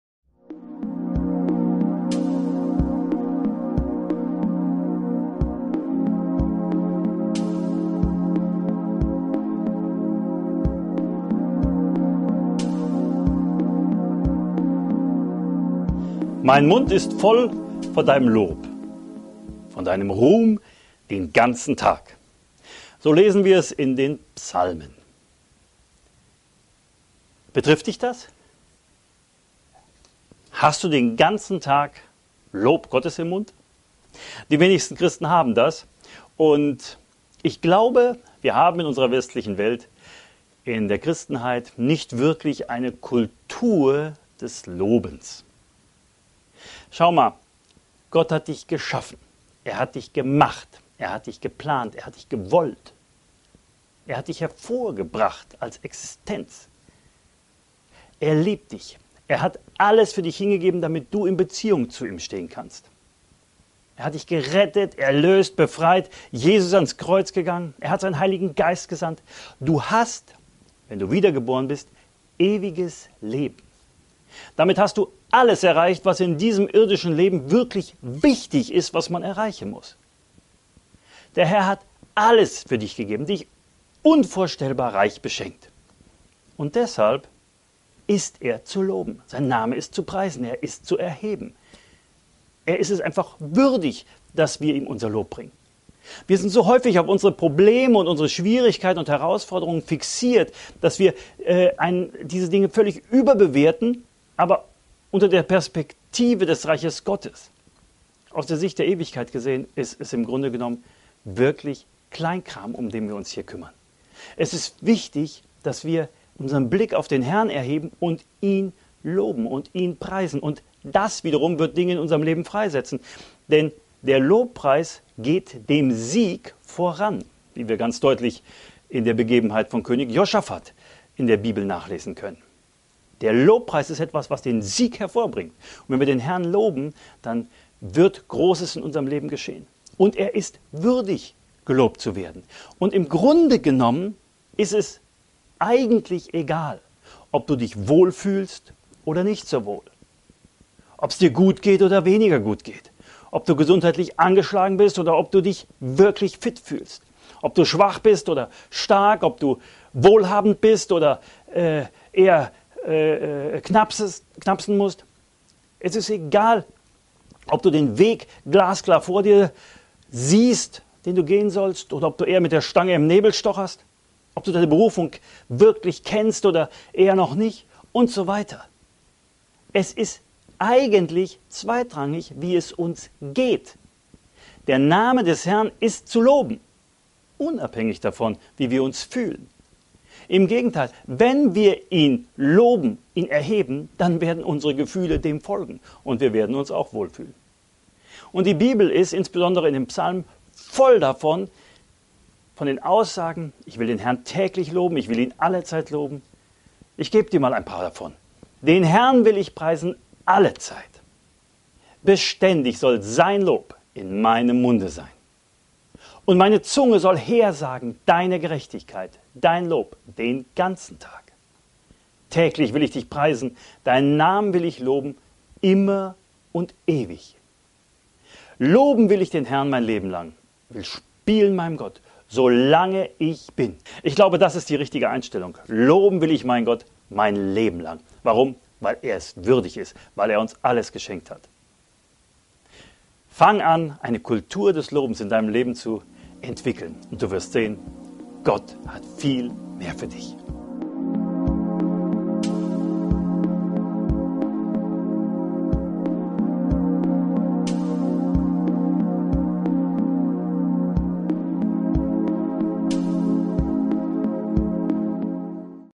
Lobe ihn in jeder Situation
Der Herr hat dich geschaffen, gerettet, erlöst, befreit, dir ewiges Leben gegeben. Er hat absolut alles für dich gegeben und dich unvorstellbar reich beschenkt. Durch seine Gnade bist du eine völlig neue Kreatur. Deshalb ist er es wert, gelobt zu werden. Und zwar völlig unabhängig davon, wie deine jeweilige Situation aussieht.

Es ist im Prinzip egal, ob du Mangel hast oder Überfluss,
ob du gesundheitlich angeschlagen bist oder dich super fühlst,
ob du einsam bist oder von Leuten umgeben,
ob du  jung und frisch oder alt und klapprig bist,
ob du schwach oder stark bist,
ob dein Weg klar erkennbar ist, oder alles um dich herum im Nebel liegt,
ob du dich unbedeutend fühlst oder wichtig,
ob du arm oder reich bist ...
... lobe und preise den Herrn, denn er ist es wert. Und zwar immer, allezeit und überall.

Entwickle in deinem Leben eine „Kultur des Lobens" und du wirst sehen, wie sich die Dinge schon bald zu verändern beginnen.

Mein Mund ist voll von deinem Lob, von deinem Ruhm den ganzen Tag.
Psalm 71,8

Den HERRN will ich preisen allezeit, beständig soll sein Lob in meinem Munde sein.
Psalm 34,2

Und meine Zunge soll hersagen deine Gerechtigkeit, dein Lob den ganzen Tag.
Psalm 35,28

Täglich will ich dich preisen, deinen Namen will ich loben immer und ewig.
Psalm 145,2

Loben will ich den HERRN mein Leben lang, will spielen meinem Gott, solange ich bin.
Psalm 146,2